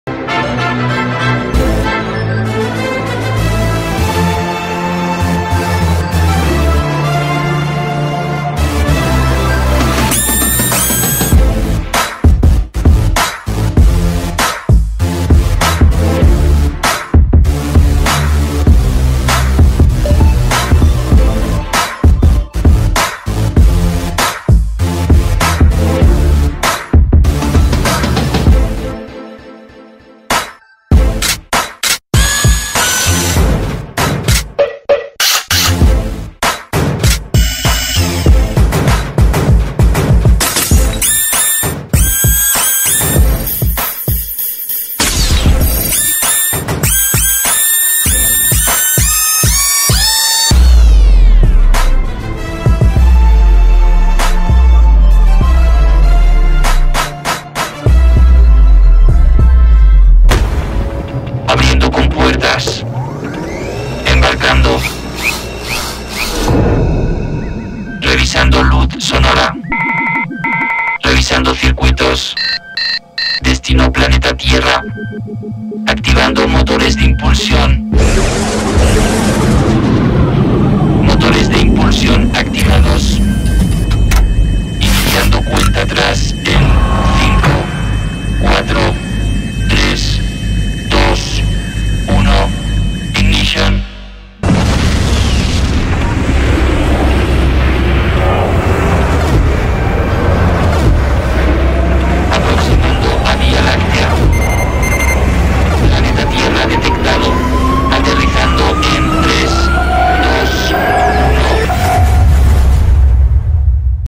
Electro Mutoid Performance
Sensacional espectáculo que integra un sistema luminoso de control remoto activando la iluminación de los vestuarios acorde con la música. Un espectáculo que demuestra que los robots también saben bailar.